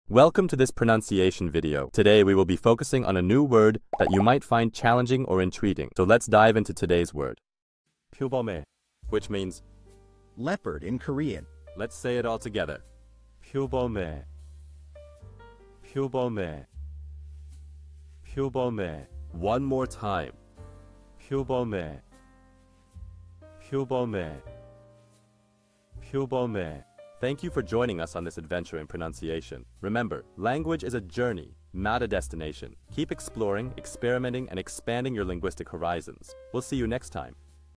How to pronounce pyobeomui | 표범의 (Leopard in Korean)
Master the Pronunciation of 'pyobeomui | 표범의' - which means : Leopard in Korean 📝 with @PronunciationKorean 🎓🗣️ - [Your Guide to Korean]

Learn to pronounce 'pyobeomui' perfectly in Korean with our easy and engaging tutorial by @PronunciationKorean! 📖📽️ In this comprehensive video, we provide you with native speaker examples 🗣️👂 and phonetic breakdowns 🎵 to ensure you get the pronunciation just right.